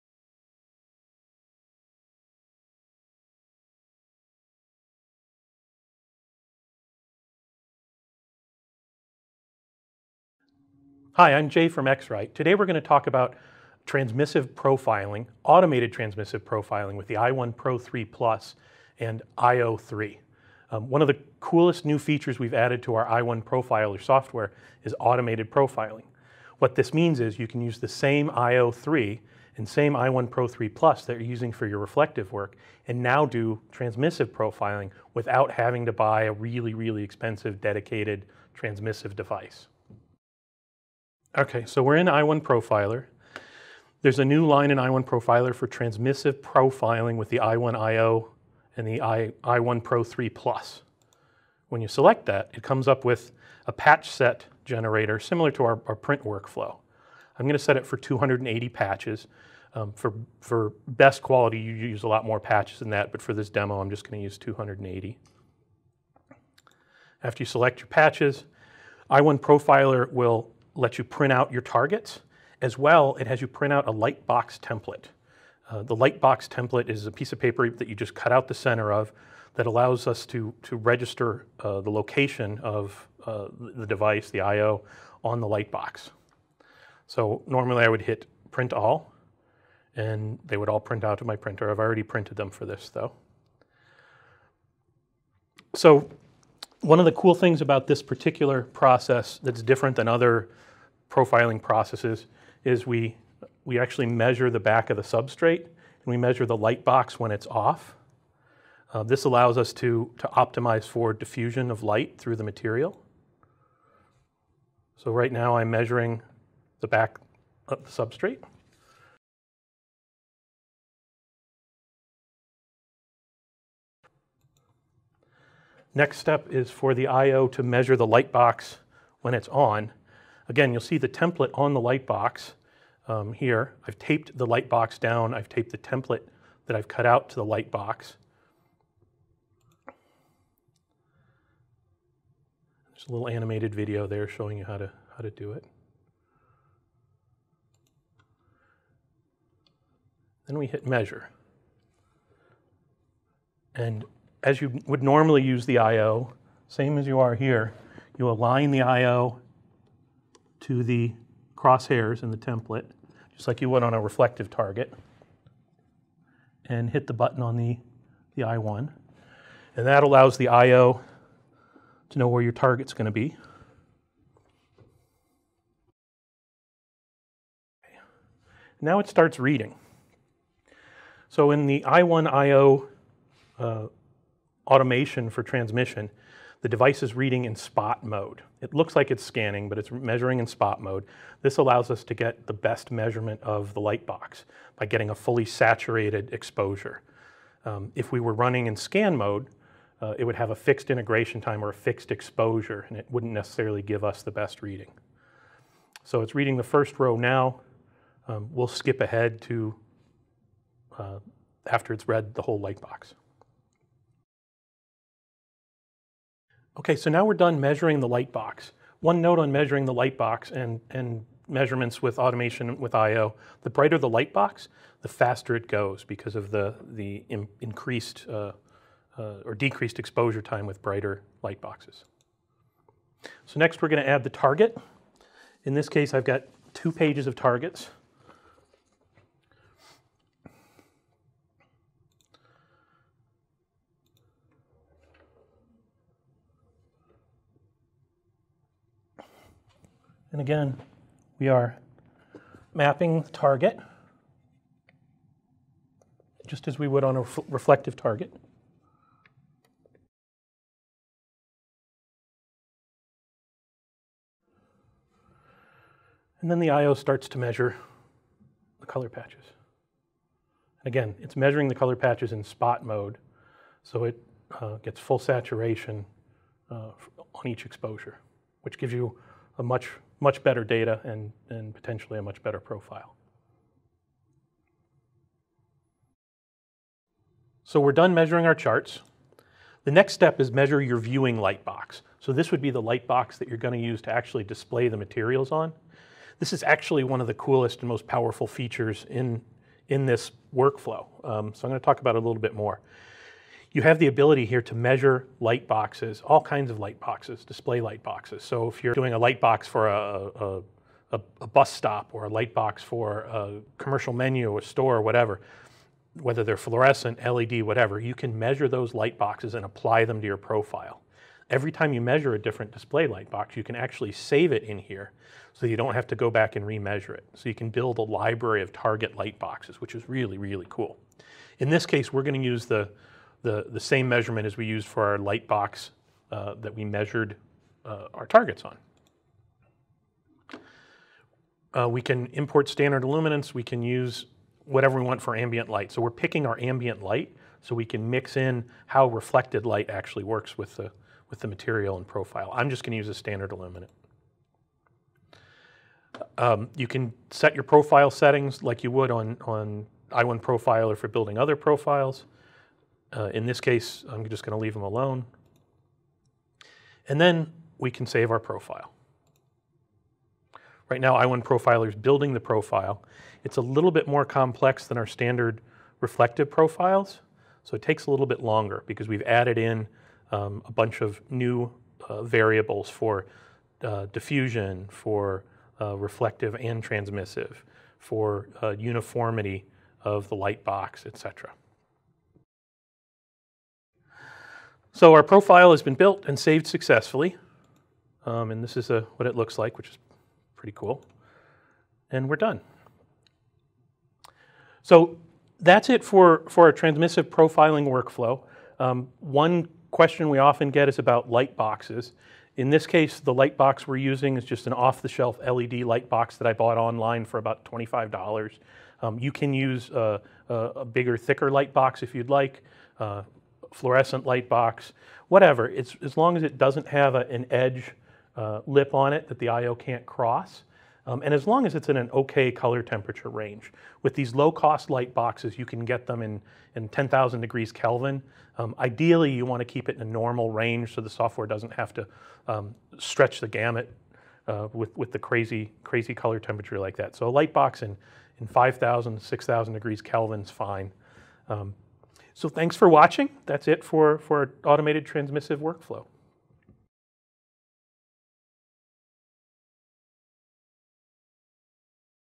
How to Automate the Profiling of Backlit Materials with an i1Pro 3 PLUS and i1iO
Product Manager for the i1Family, Jay Kelbley demonstrates automated transmission profiling for the i1Pro 3 PLUS and i1iO using i1Profiler v3.3 software. With the combined iPro 3 Plus with i1iO table, wide-format, signage and industrial printers can quickly and easily automate the creation of ICC profiles for film, textiles, vinyl, paper and other materials used in backlit signage applications.